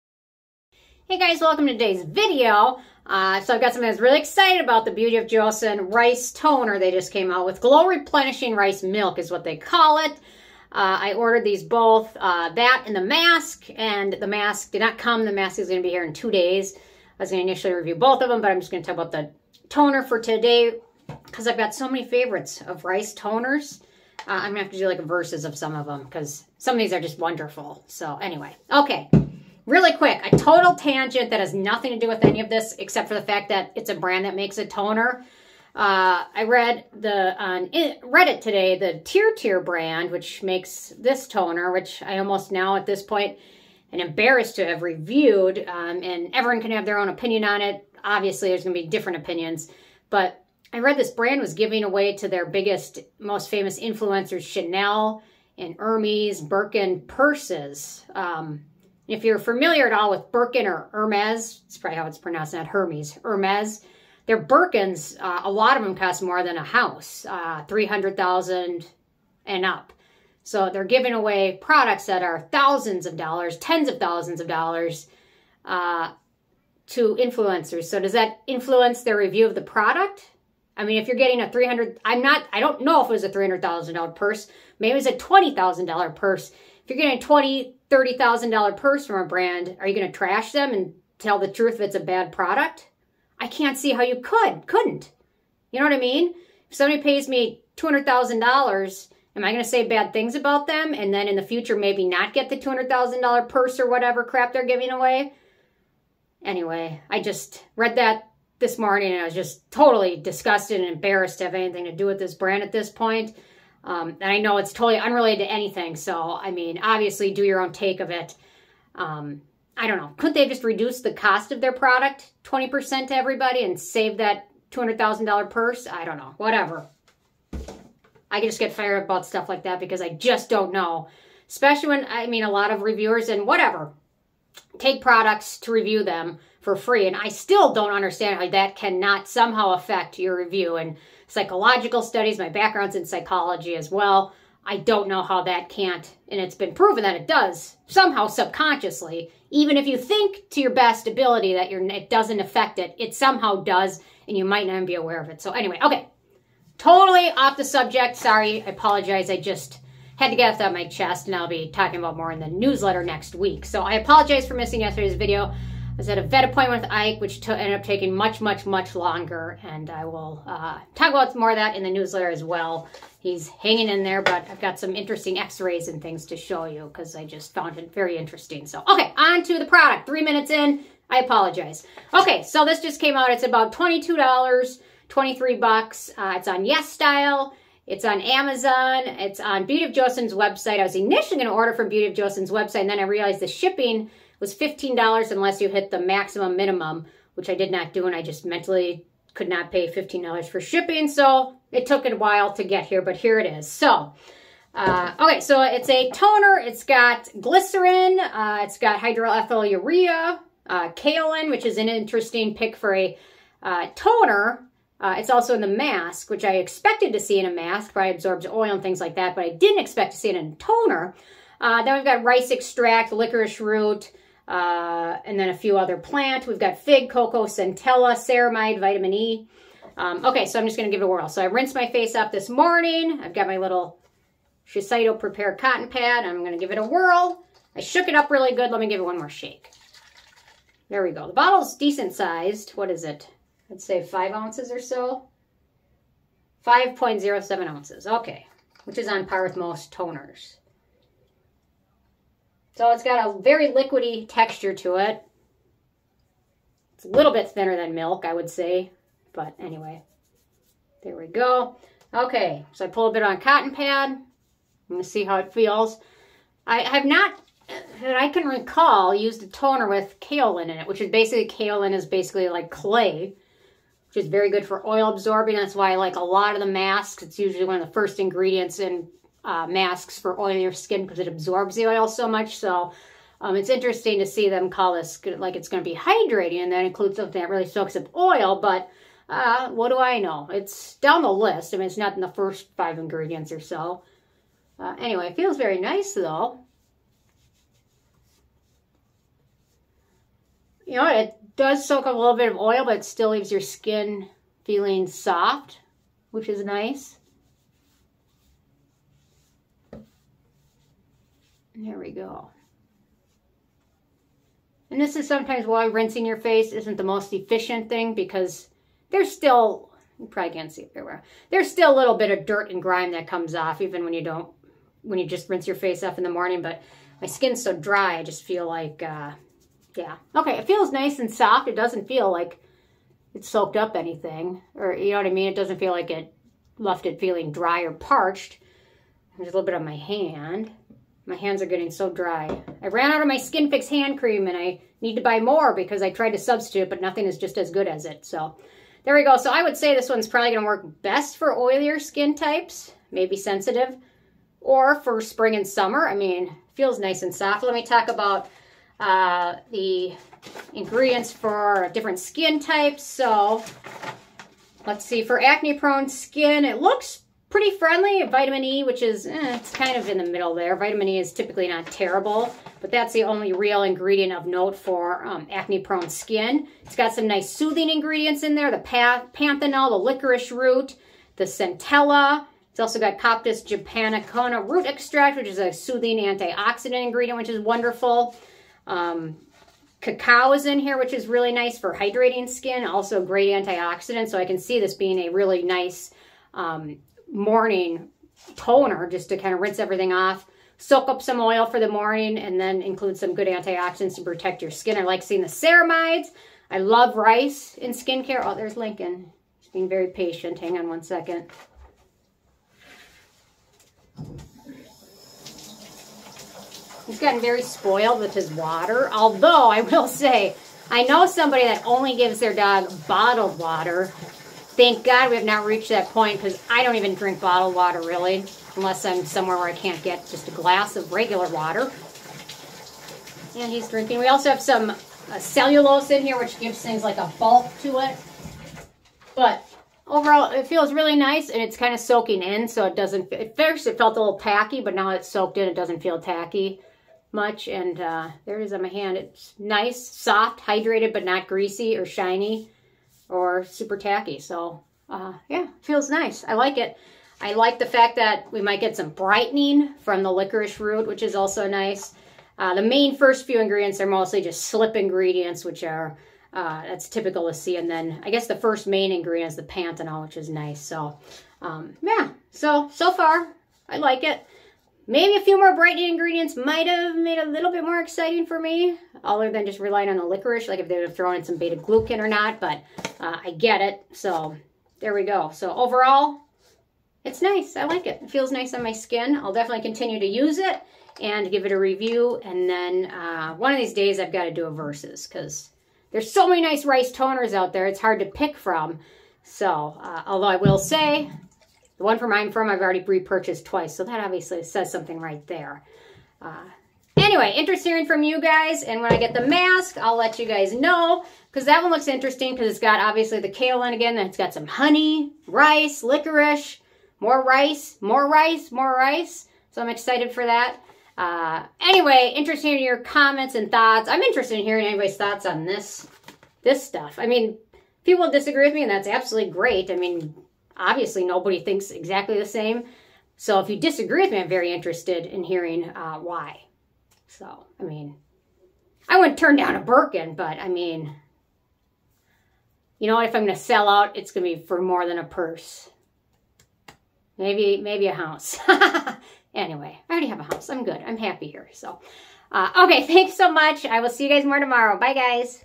Beauty of Joseon New! Glow Replenishing Rice Milk Toner First Impressions Review
Beauty of Joseon Ground Rice and Honey Glow Mask, $23
Use Rewards Code NOBSBEAUTY for an additional discount!

At Amazon
(sold & shipped by Beauty of Joseon)